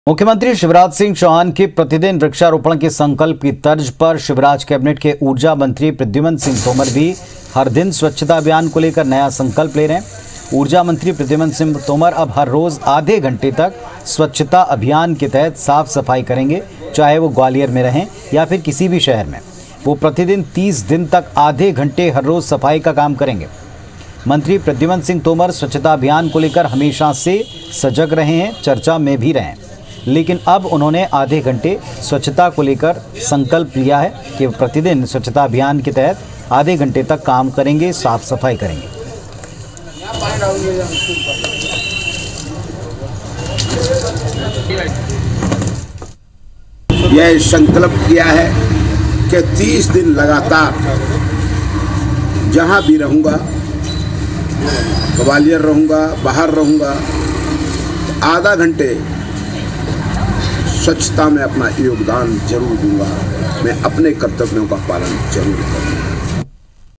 स्वच्छता अभियान को लेकर Minister Pradhuman Singh Tomar का संकल्प। प्रतिदिन 30 मिनट करेंगे साफ-सफाई
IBC24, स्वच्छता अभियान को लेकर Minister Pradhuman Singh Tomar का संकल्प, प्रतिदिन 30 मिनट करेंगे साफ-सफाई